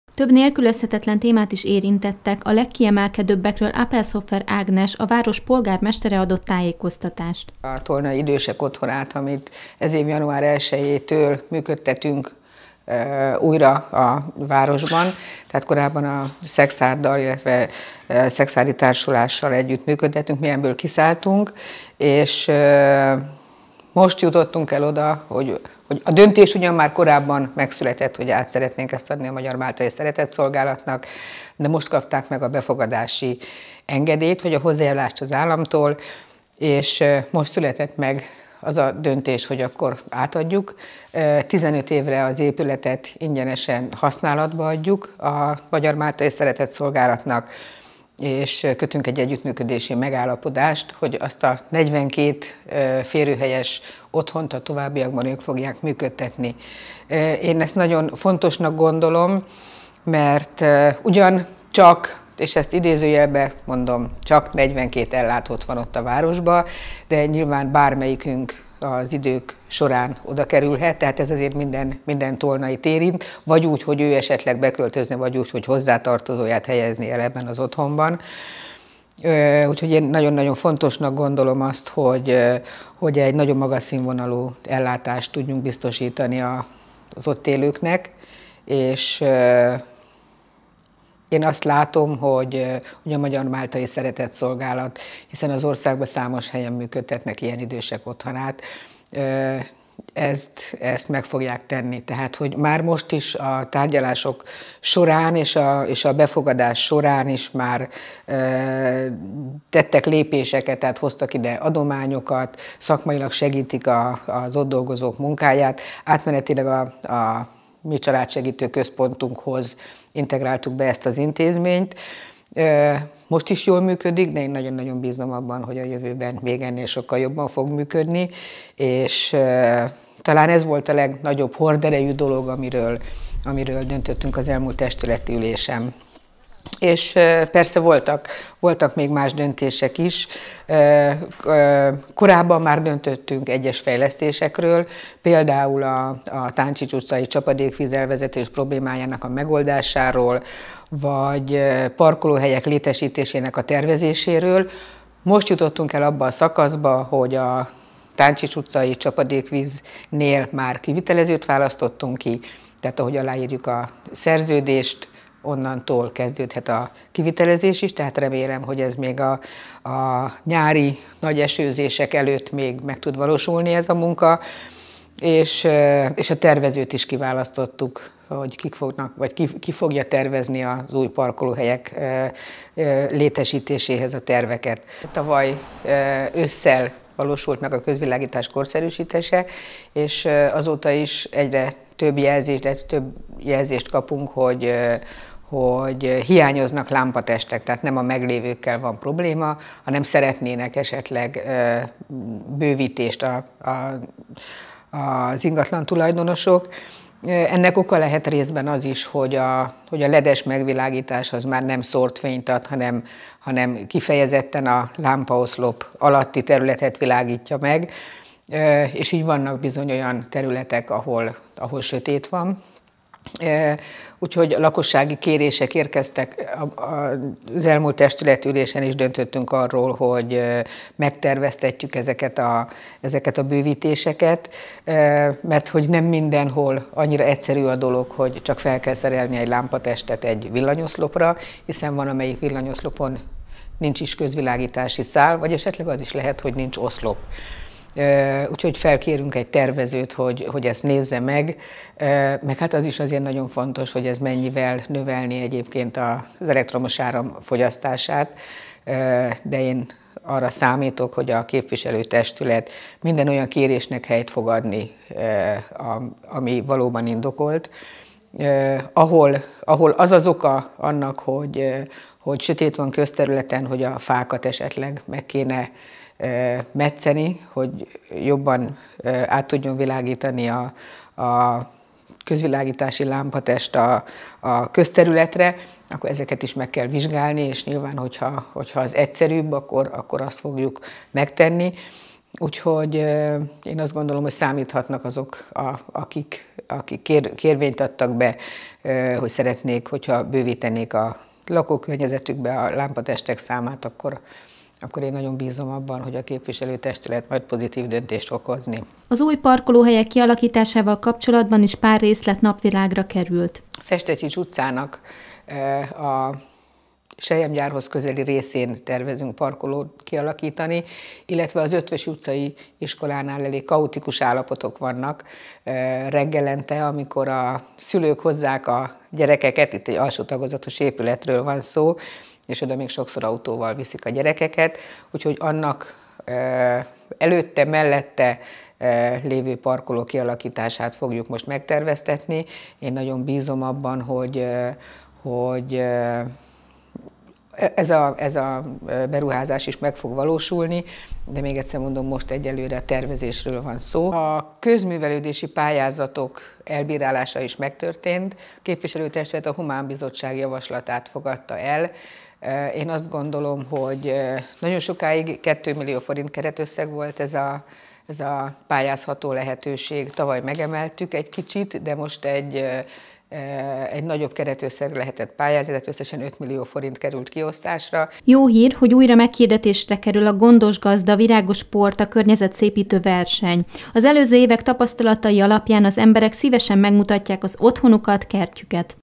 Képviselő-testületi ülés - tervekről, intézkedésekről tárgyaltak
A csapadékvíz elvezetési rendszer kiépítése, és új parkolóhelyek létesítése is napirendre került a testületi ülésen.